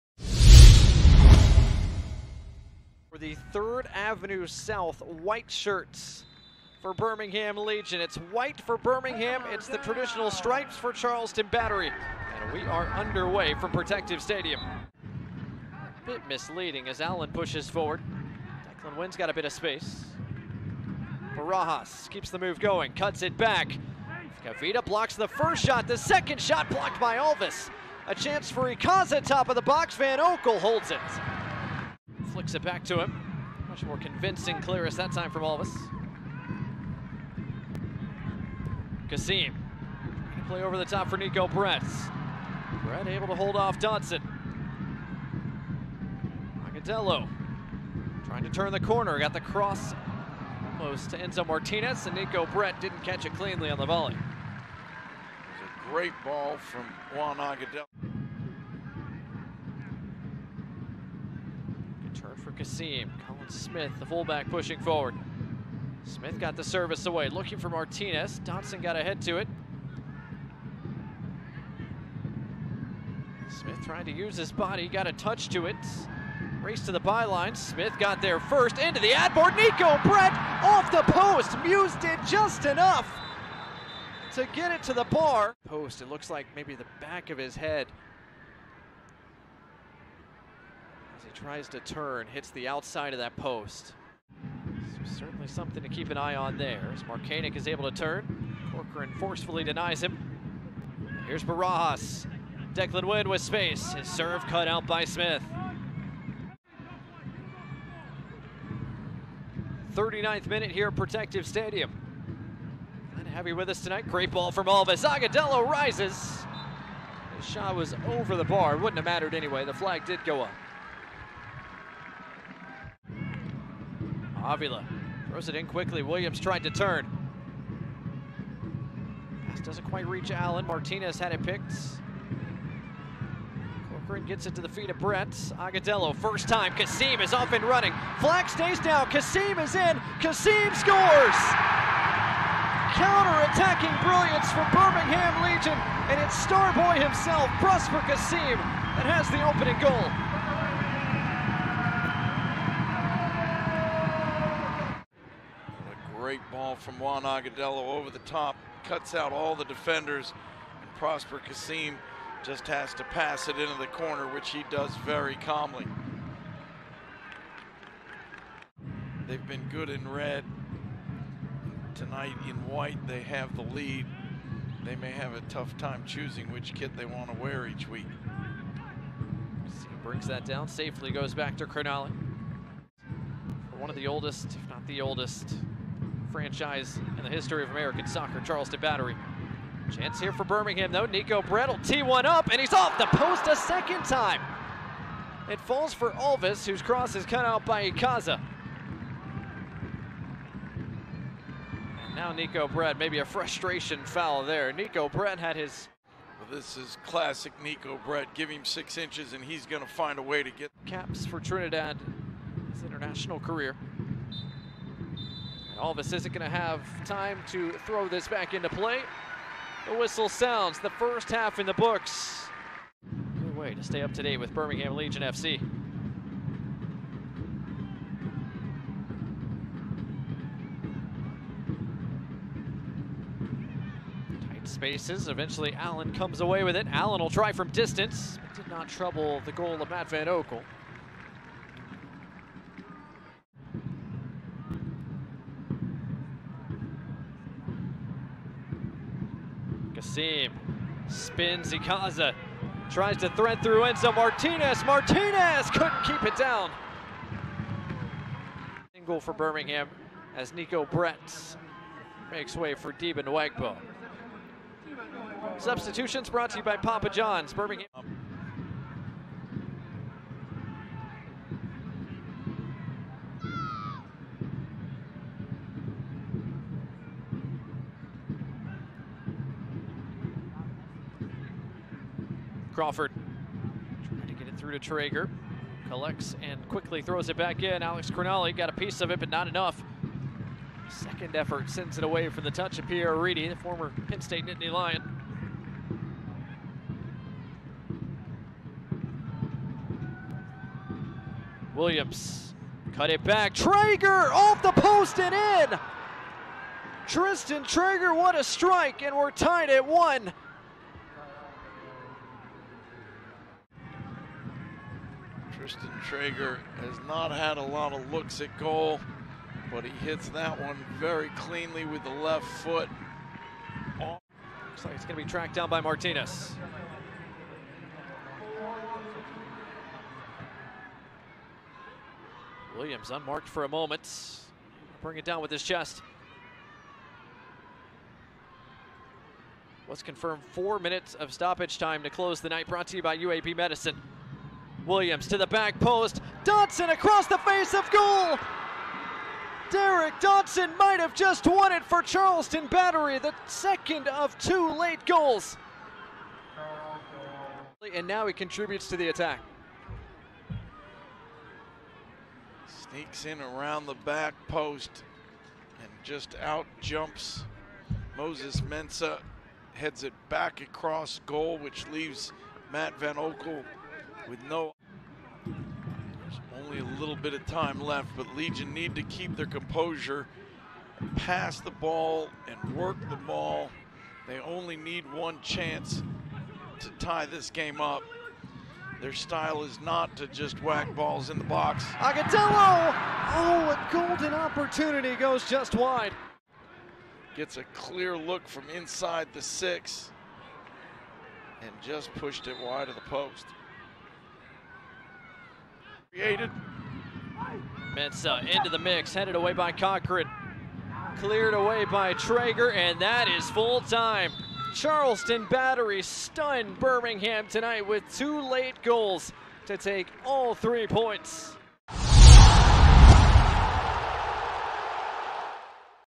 7 29 2023 Birmingham Legion FC vs Charleston Battery Game Highlights
Charleston comes to Birmingham on 7.29.23.  Highlights from Protective.